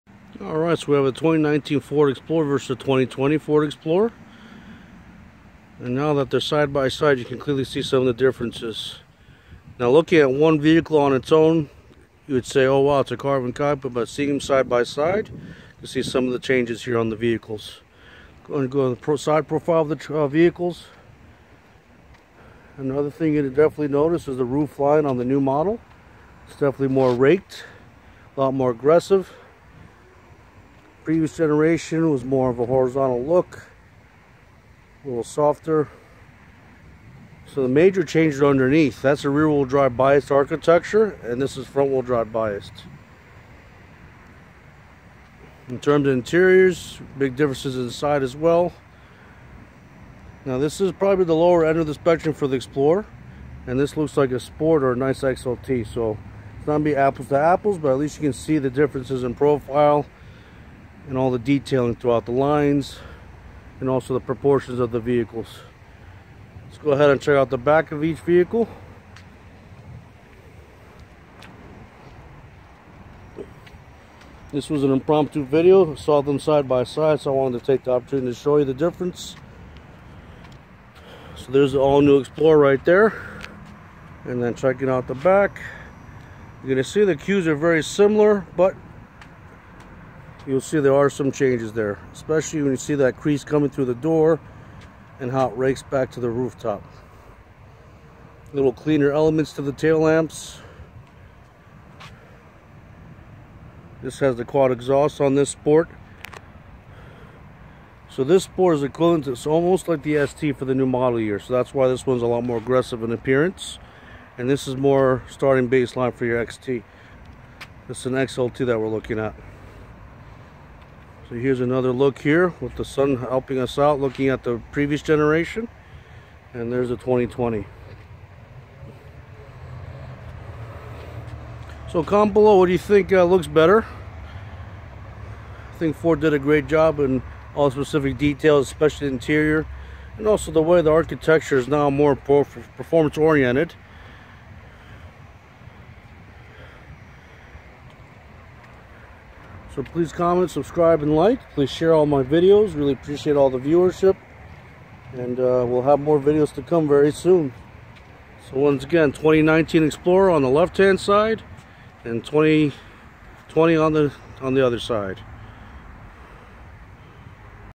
2019 Ford Explorer vs 2020 Ford Explorer - Checking out the exterior changes
Ford revamped the explorer for 2020. Here is a side by side look at the changes from 2019 to 2020. Which one do you all prefer? Please share your opinions.